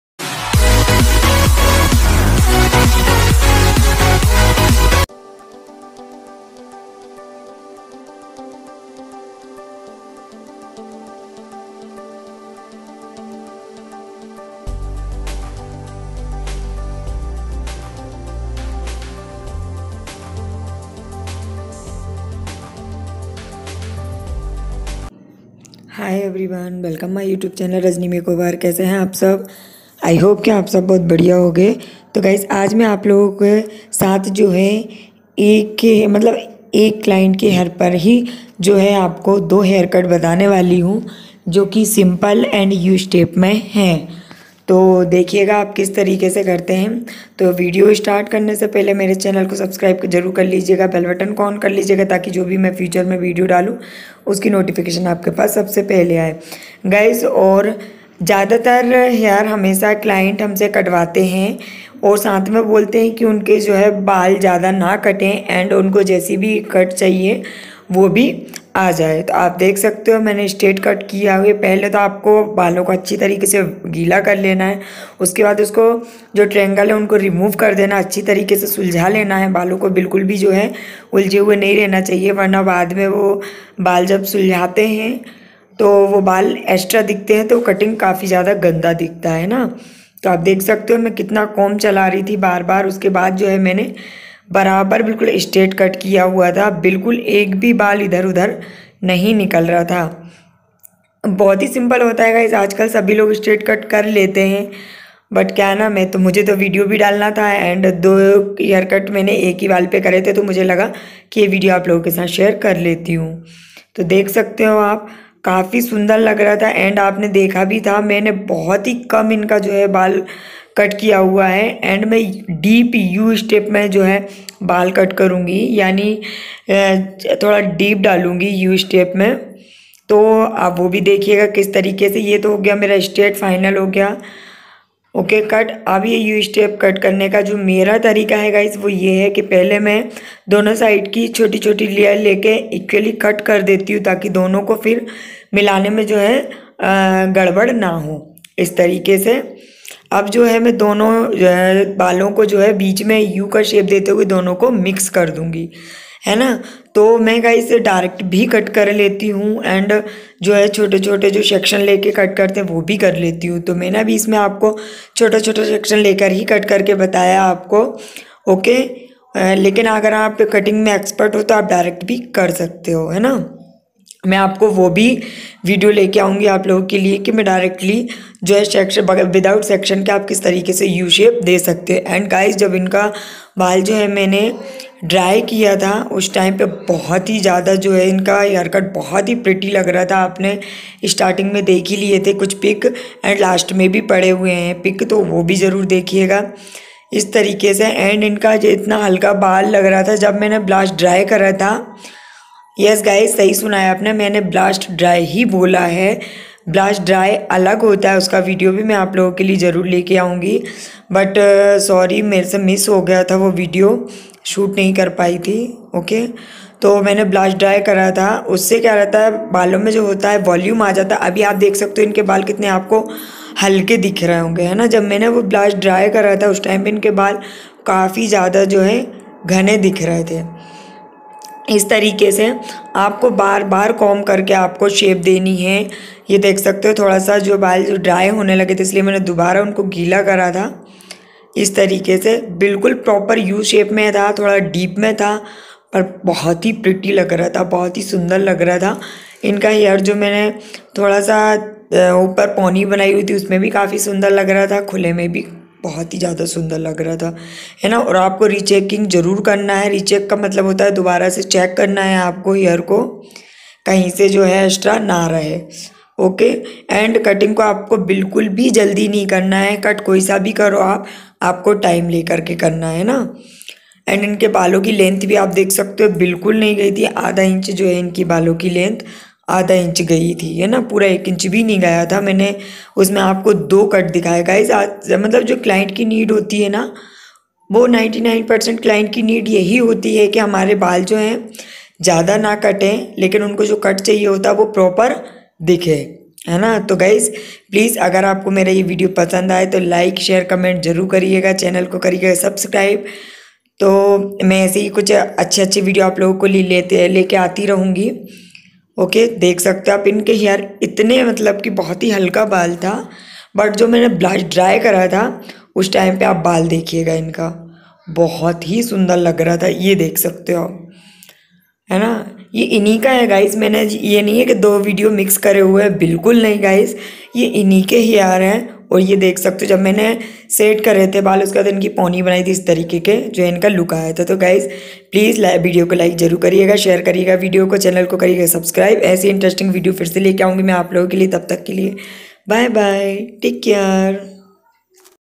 स्टेट कट विथ U कट कैसे कर सकते हो आसान तरीके से//Rajni makeover 💅💄
hi 👋 everyone welcome my YouTube channel Rajni makeover 💅💄

AJ Ka Mera yah video hair se related hai ki ap kis tarike se stet cut with U cut Kar sakte ho bahut hi easy tarike se to agr apko Mera yeh video halpfull lage to plz

Rajni makeover 💅💄